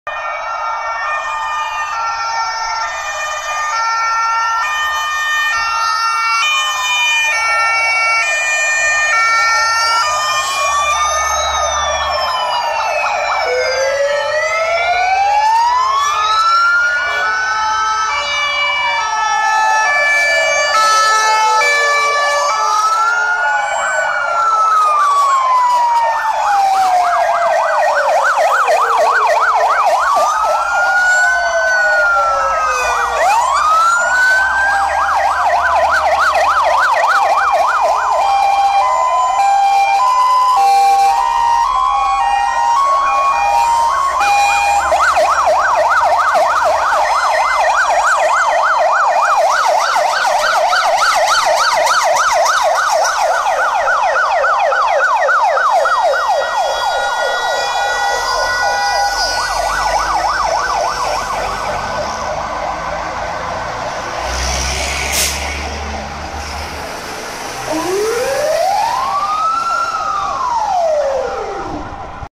Przejazd kolumny pojazdów PSP i OSP z okazji Dnia Strażaka Pabianice 04.05.2015.
Przejazd alarmowy kolumny pojazdów PSP i OSP z powiatu pabianickiego z okazji Dnia Strażaka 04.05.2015 rok :)